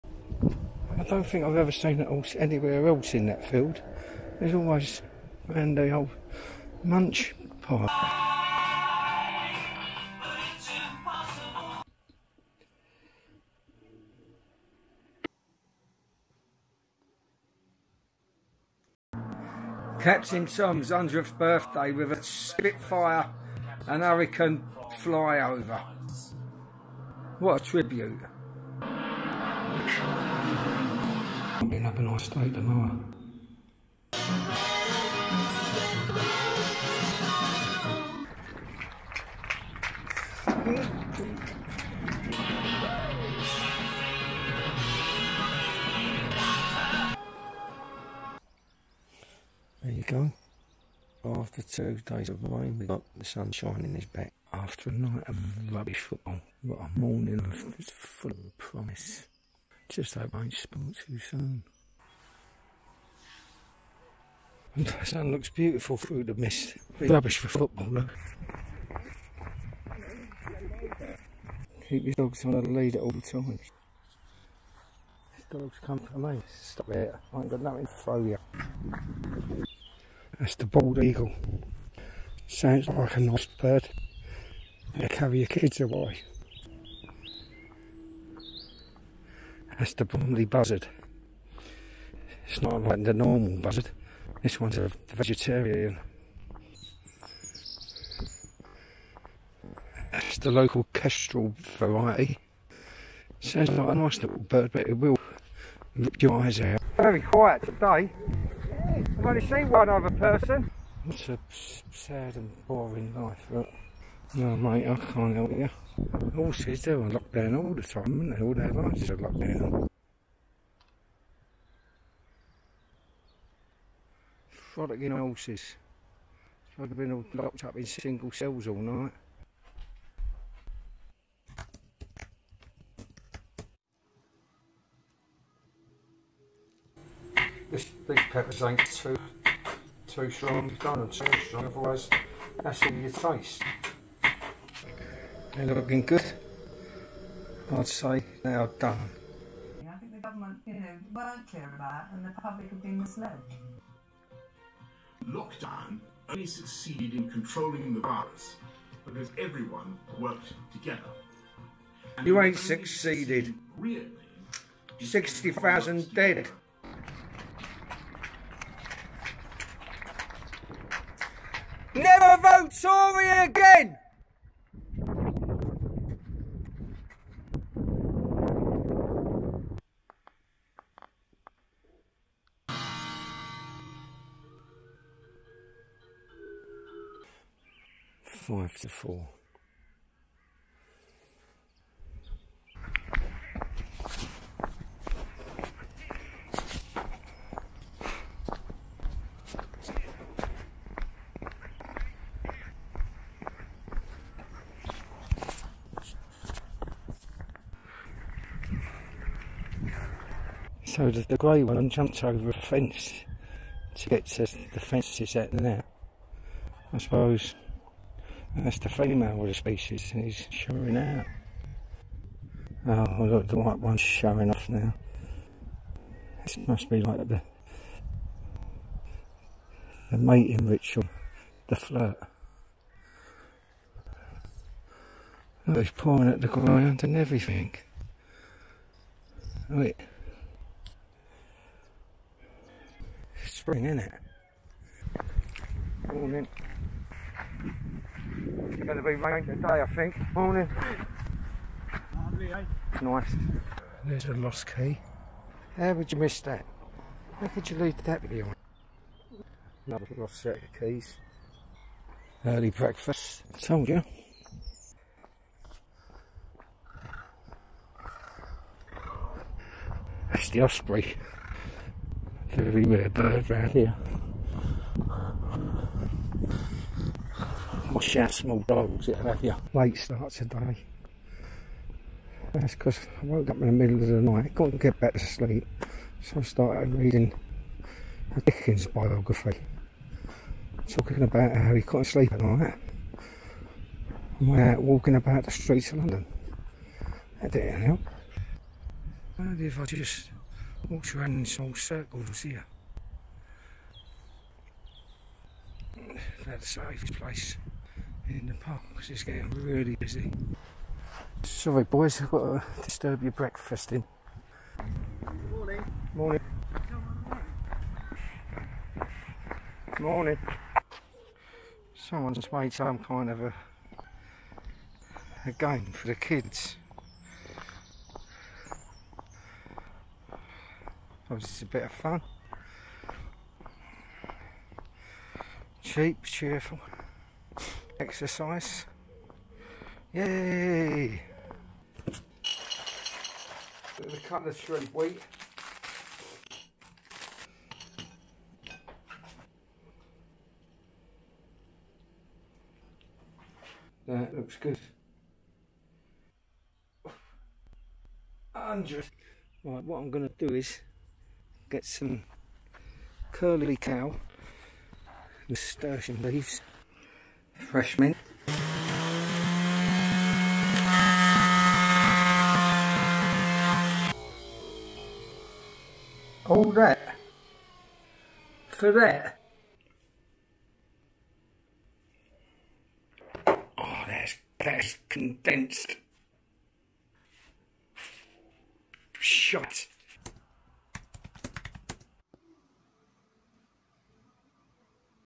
Lockdown Routines Pt 2
When life is on lockdown it is easy to fall into simple routines to keep busy and stay safe. But those mundane routines throw up unexpected thrills like beautiful sunrises, wonderful birdsong and strangers who say good morning at 6.30am. Nature returns to being the most important thing in your life and becomes the reason you get up early and go out with a camera.